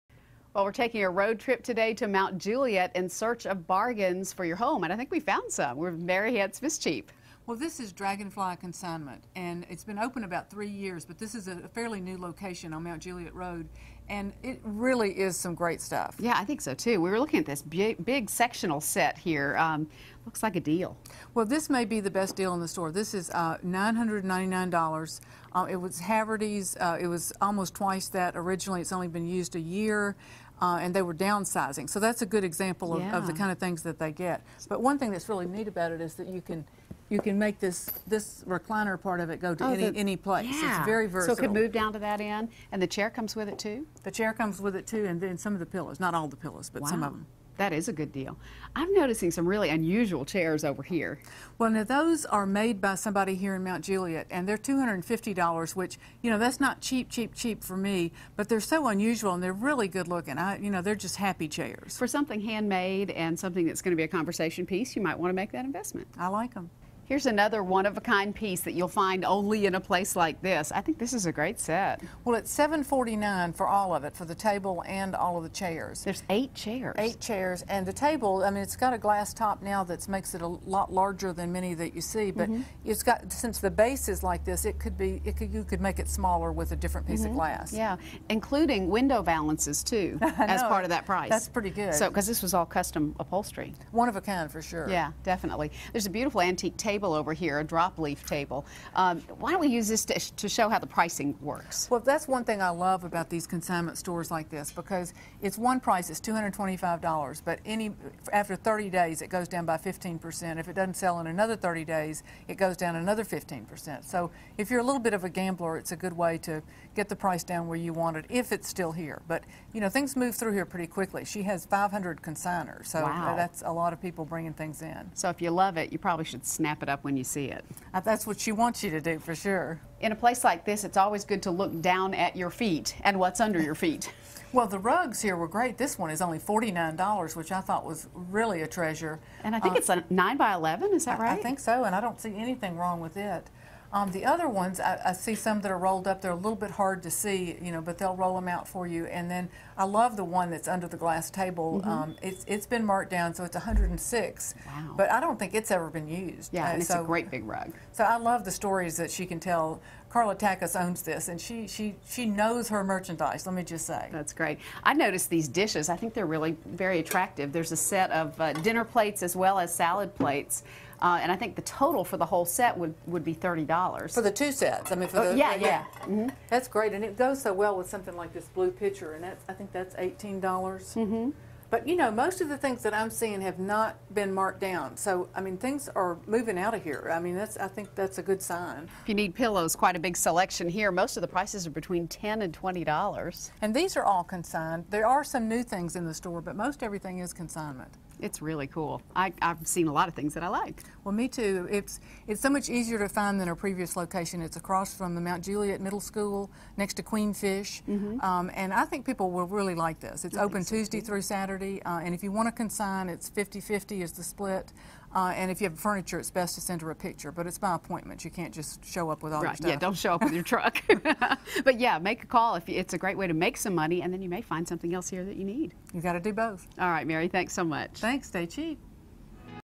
Ms. Cheap's Shopping Excursion To Dragonfly Consignment Boutique
Ms. Cheap takes us to Mt. Juliet for bargains on furniture, decor and more